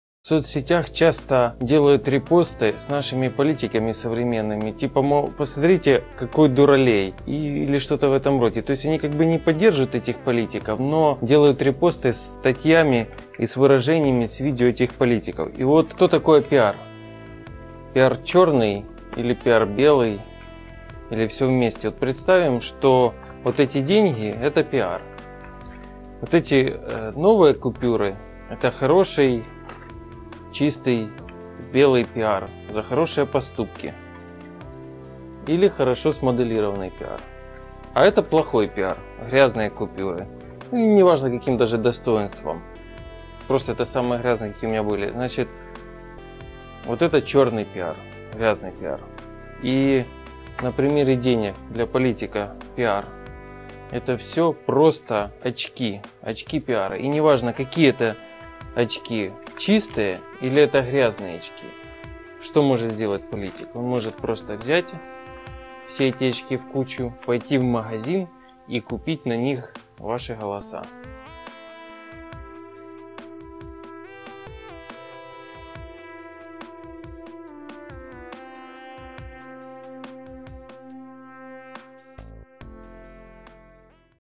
Как черный пиар политиков превращается в ваши голоса
Рассказывает Артем Селезнев
Суть посыла:
Не пиарте, не рекламируйте, не создавайте
вам политиков. Эти мрази только в выигрыше.

Присоединяйтесь и творите новейшую историю Украины
Каналы ютуб

Сергей Михайлович Гартингер -Глава Черкасской областной ячейки партии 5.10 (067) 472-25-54
Наши ссылки: